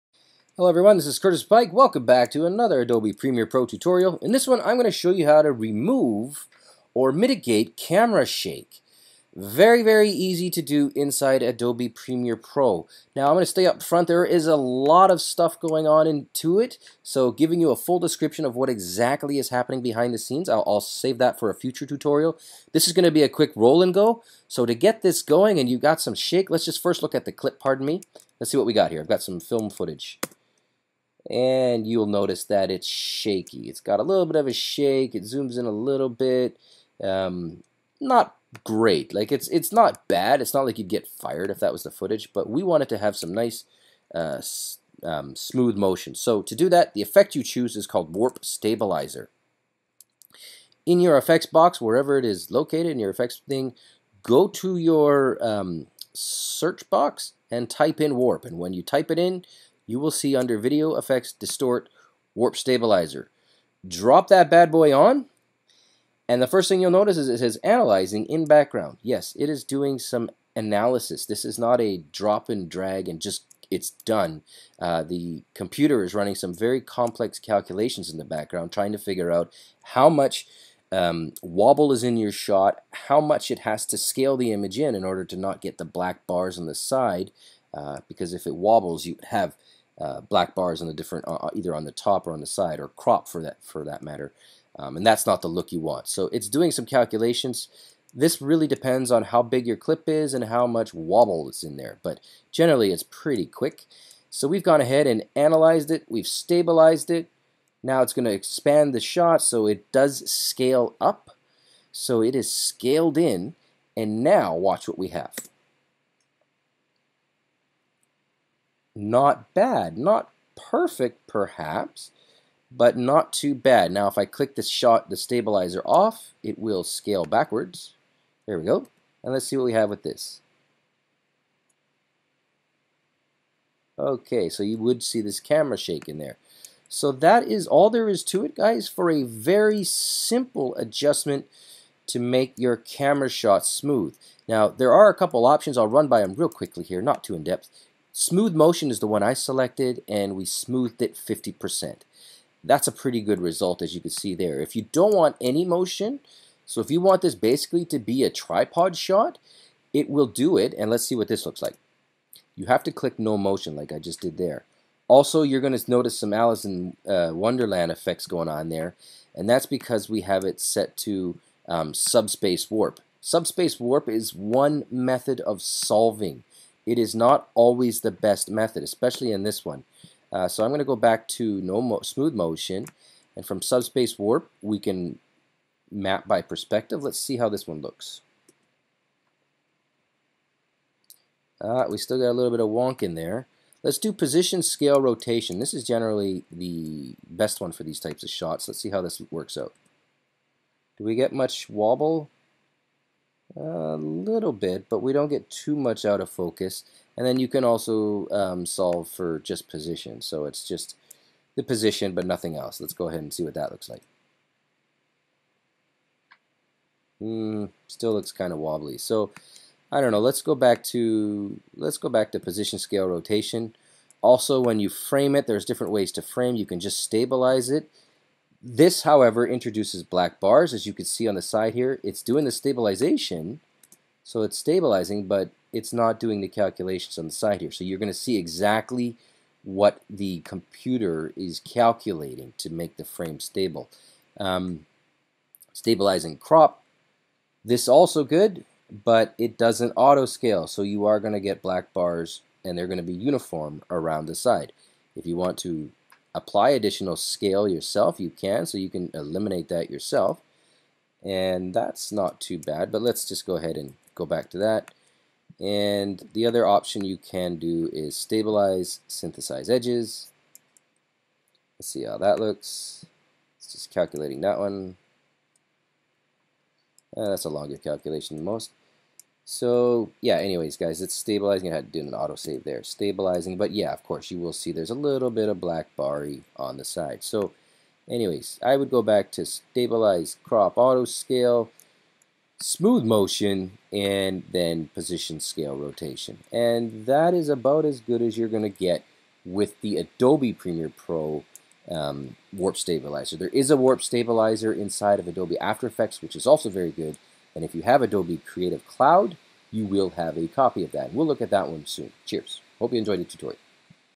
How to remove camera shake with Adobe Premiere Pro CC - Warp Stabilizer
Here is a quick video showing you how to remove camera shake from your videos.  We use the warp stabilizer effect inside Adobe Premiere Pro CC.